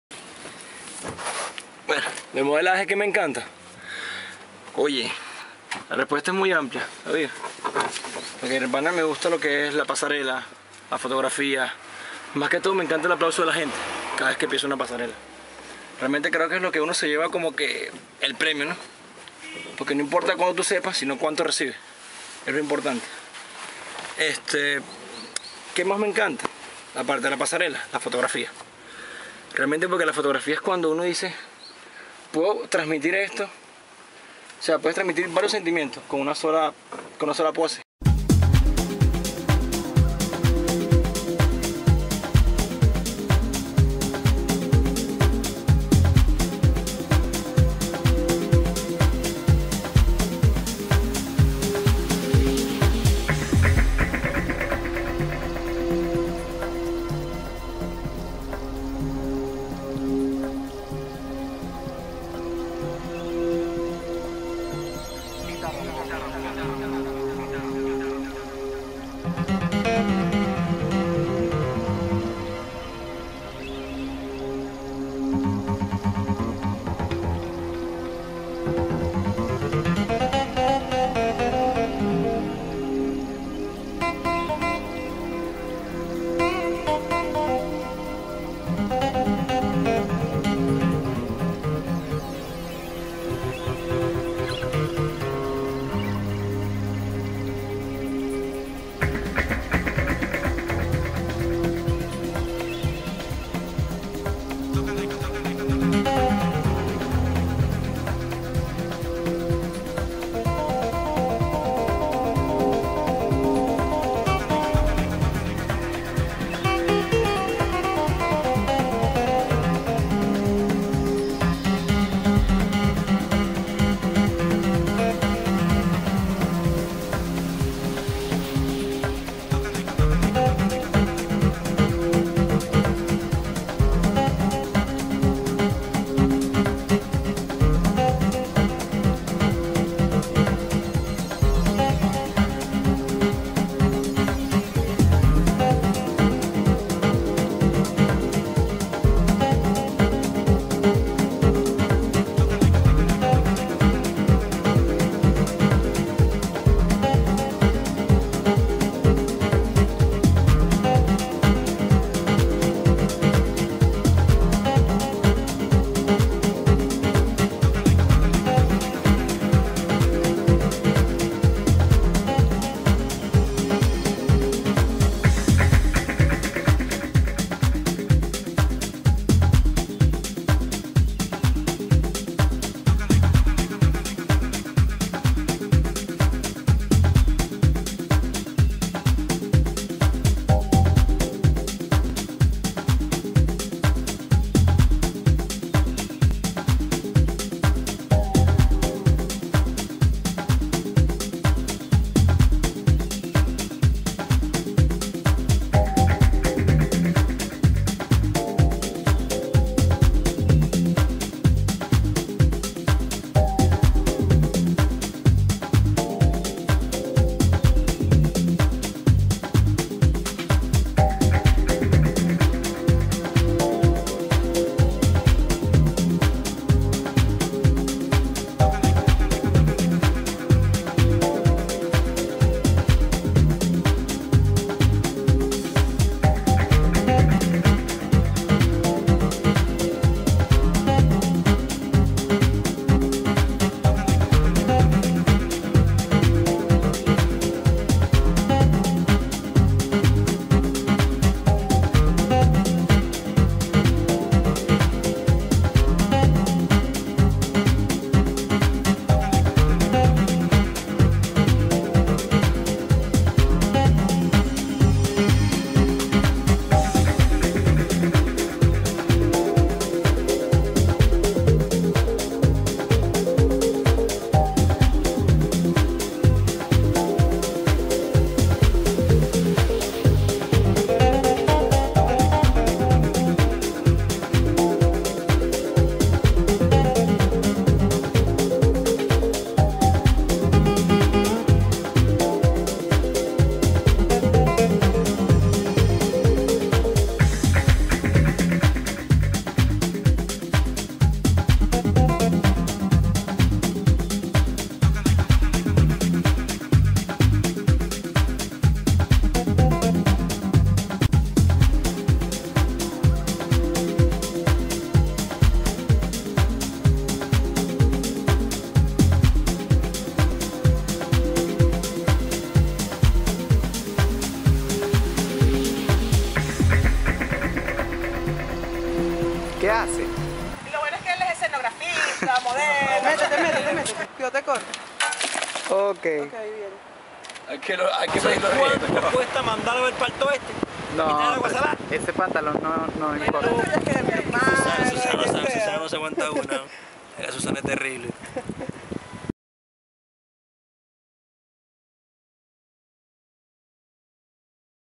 Leonardo Mujica Photo Session
Tras cámaras u entre vista de una sesión de fotos, Tras el lente de Susana Gonzales, con la Dirección de Heitters Diaz y una Producción de COFAV.